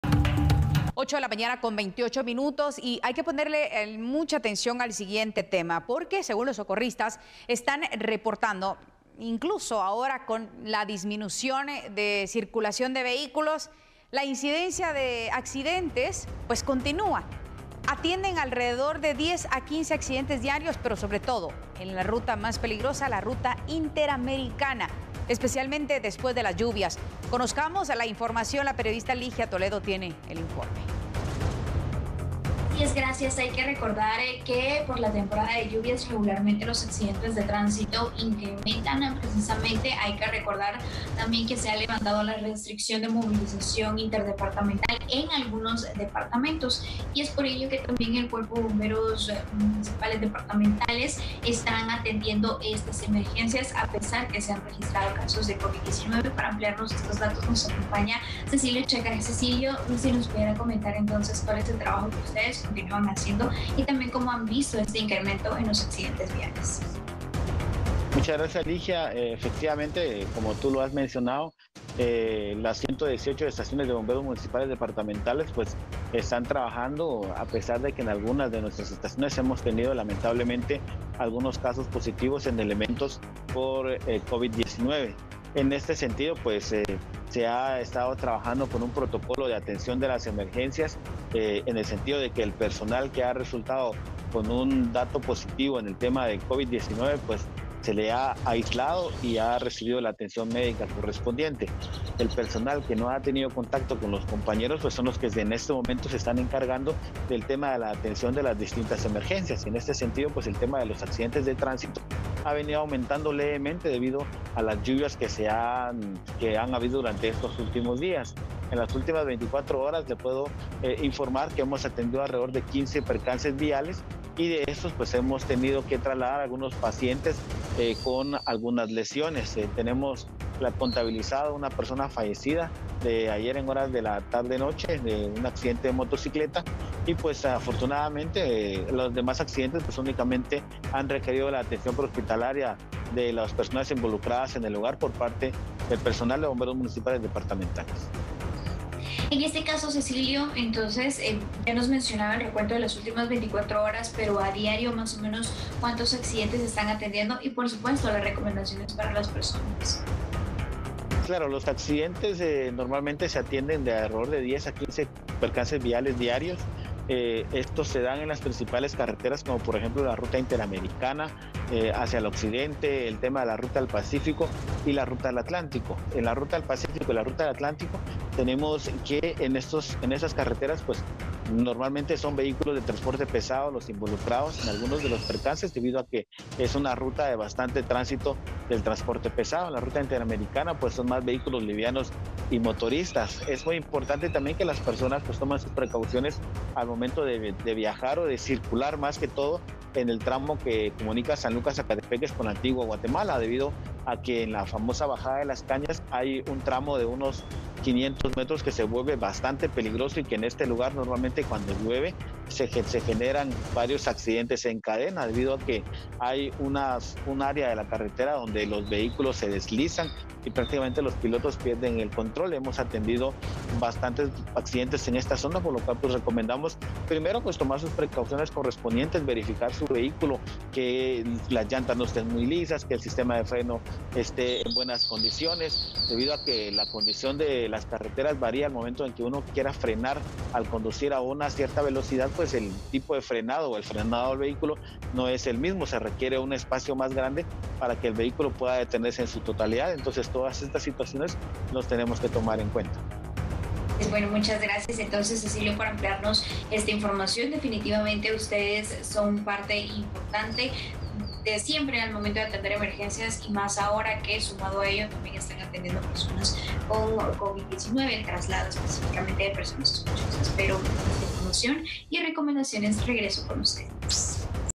Bomberos atienden entre 10 y 15 accidentes diarios
El Cuerpo de Bomberos Municipales Departamentales atiende entre diez y 15 diarios percances viales diariamente. La Ruta Interamericana se caracteriza por su alto riesgo  en época de lluvias.